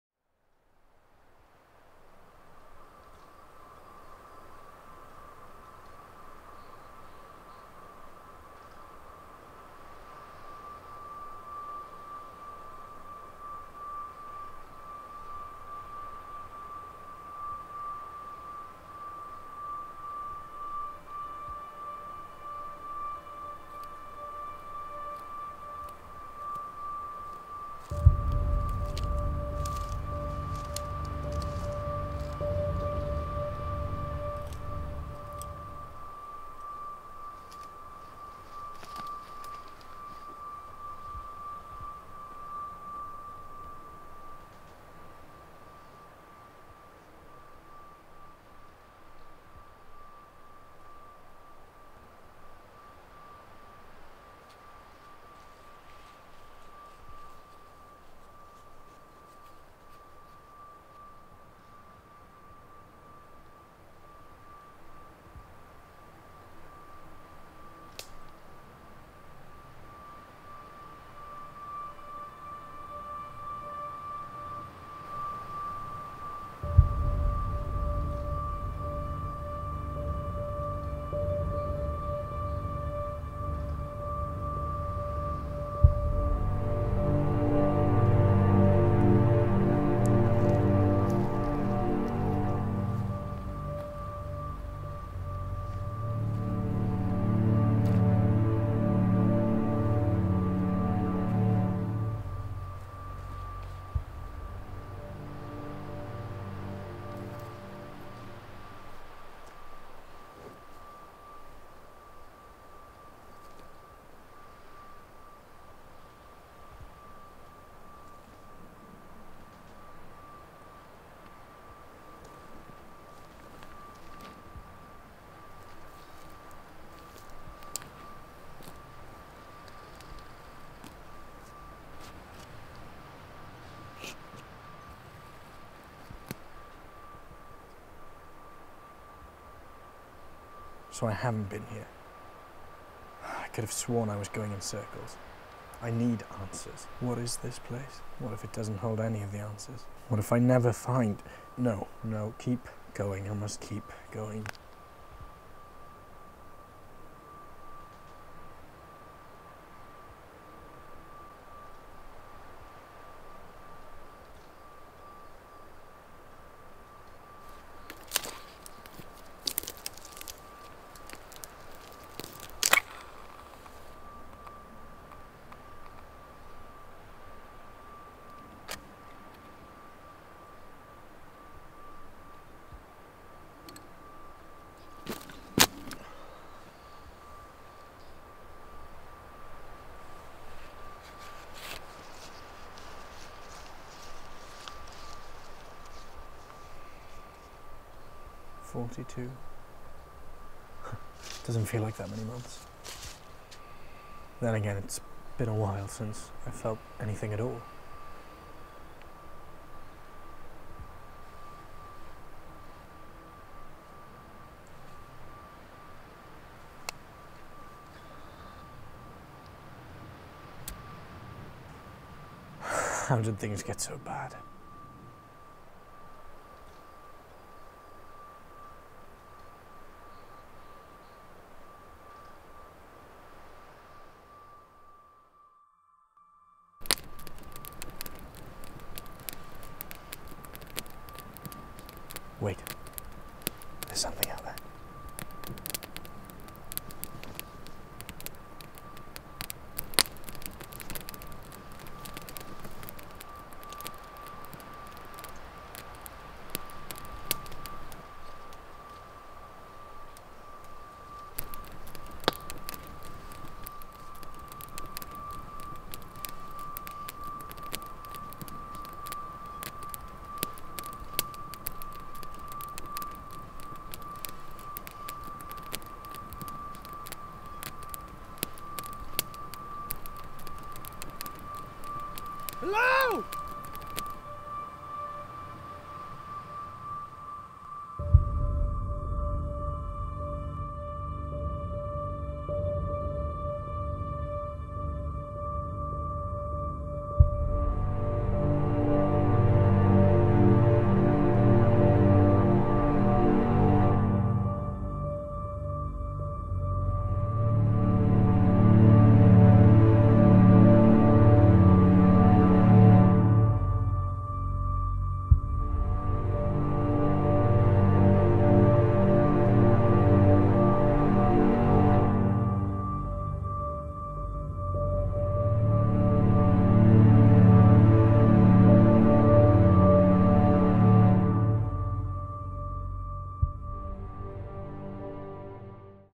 ISOLATION - short film// by Megan Jones
* PLEASE WATCH WITH HEADPHONES *

3.5 years of the Apocalypse has left John the last person on earth. 10 days into his search for the building with the answers he encounters things which question his isolation, or if it's all just in his head.

Welcome to my short film/scene that I created for my major project work for the Foundation course. It's been a stressful 10 weeks, thanks to all who supported me throughout